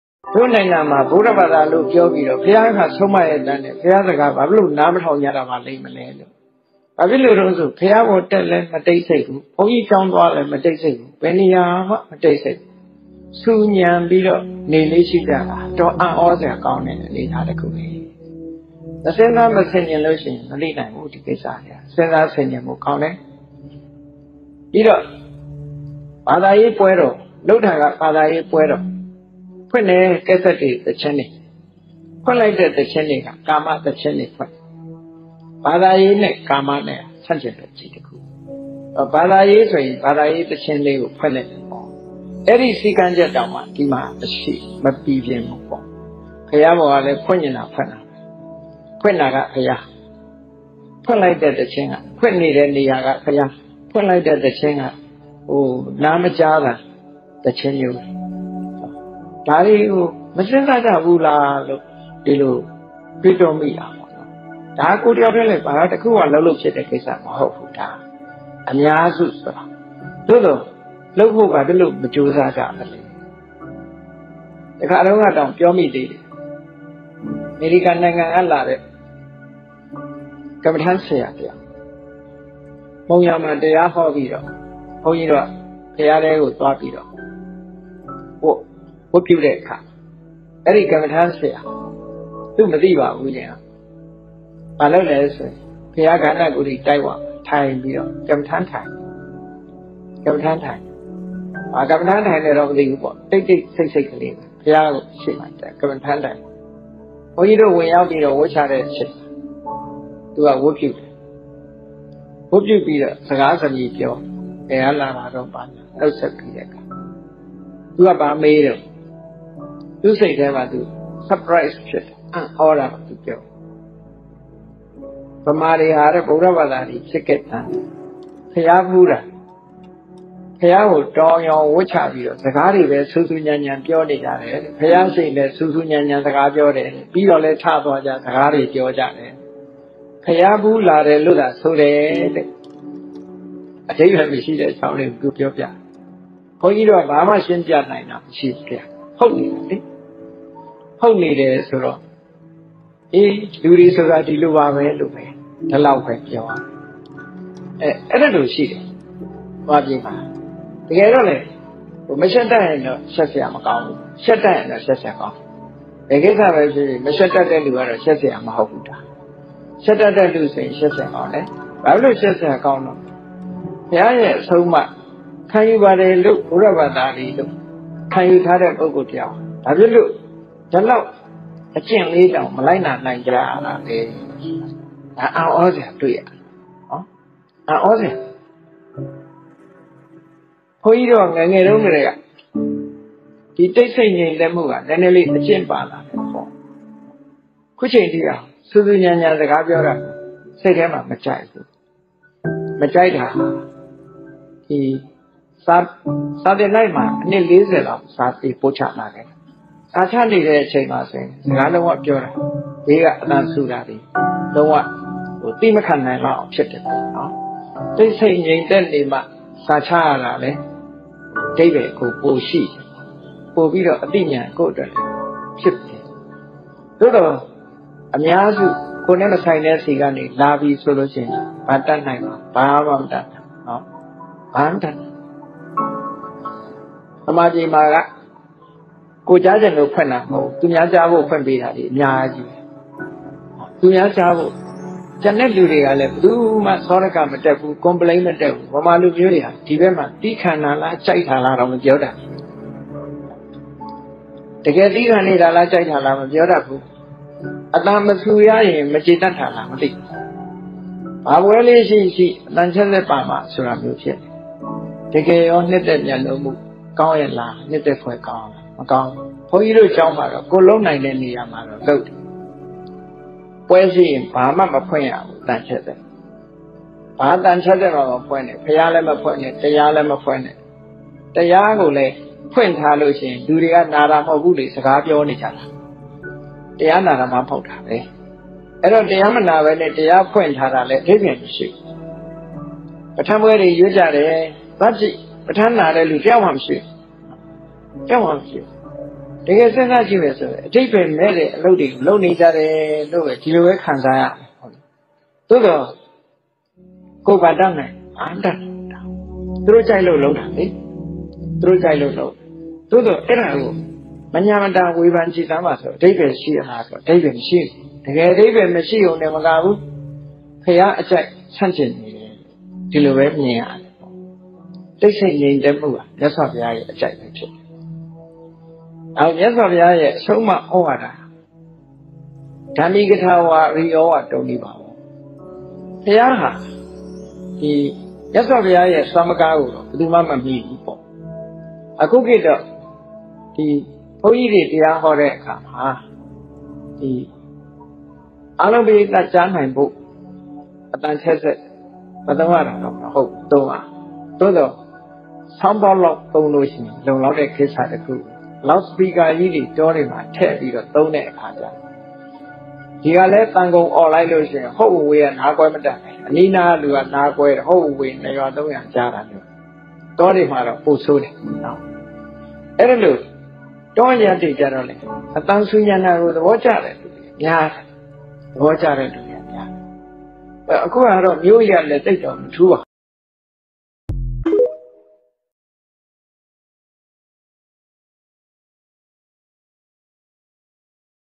တို့နိုင်ငံမှာဗုဒ္ဓဘာသာလို့ပြောပြီးတော့ဘုရားကဆုံးမရဲ့သားနဲ့ဘုရားစကားဘာဖြစ်လို့နားမထောင်ကြတာပါလိမ့်။
Content : အသံဆူညံခြင်း(@drnandamarlar)

မြတ်ဗုဒ္ဓ၏တရားတော်များကို ဤChannelမှာ နာယူပူဇော်ရင်း ကုသိုလ်တရားတွေ ဆထက်တပိုး ပွားများကြပါစေလို့